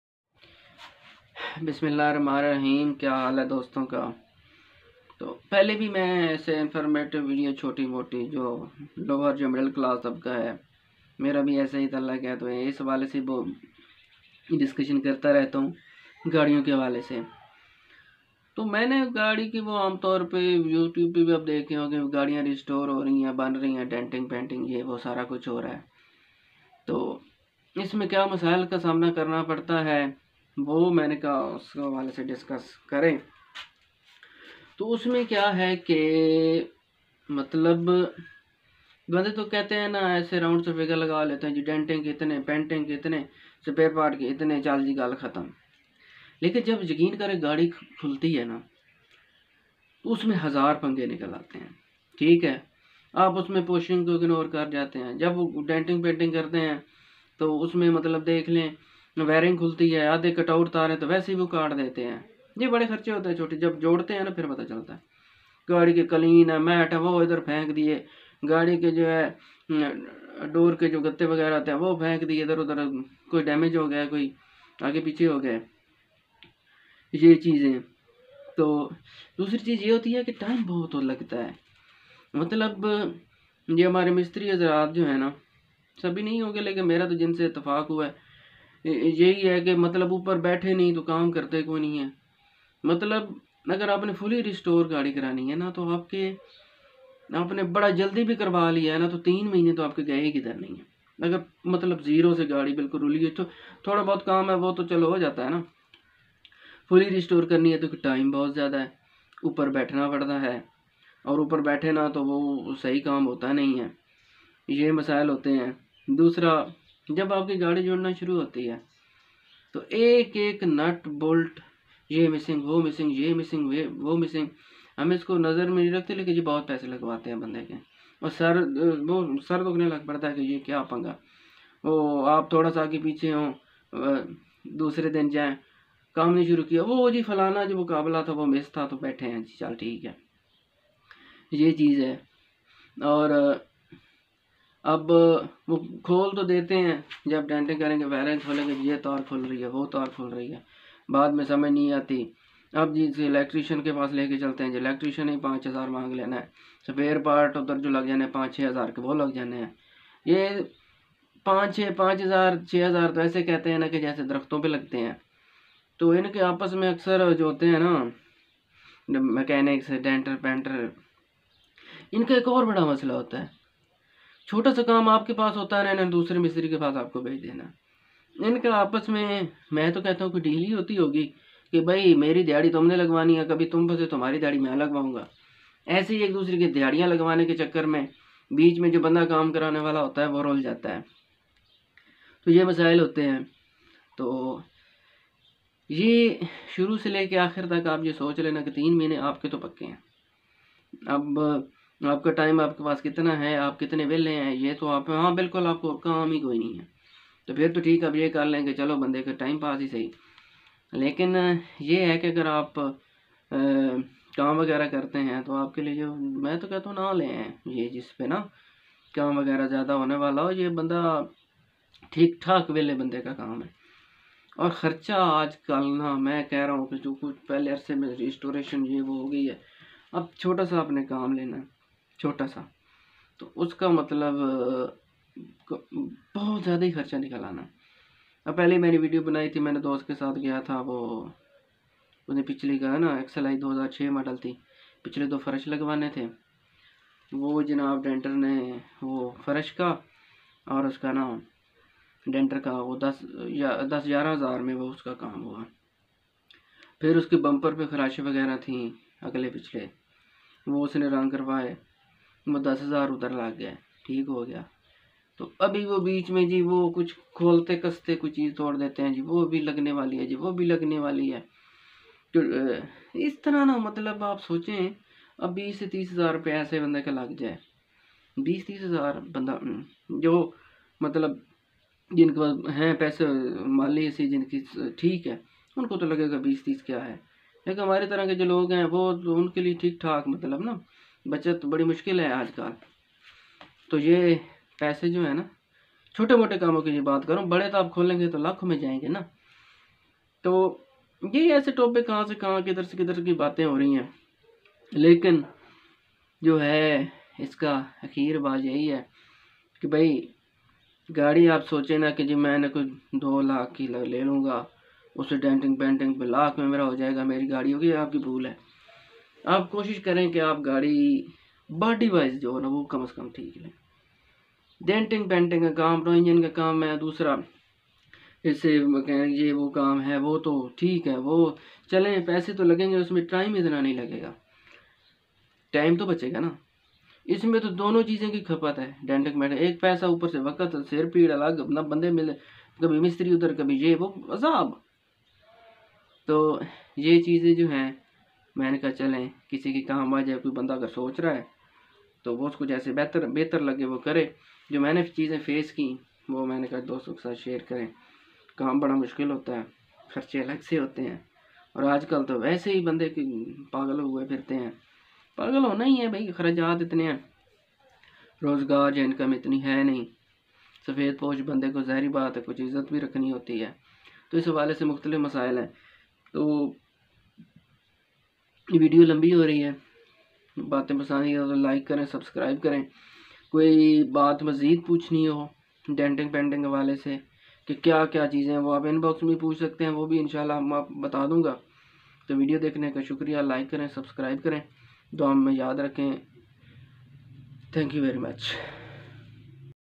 denting painting krwana azab sy Kam ni
is video ma ham discuss krain ge k denting painting krwaty wakat kin problems ko face krna prta ha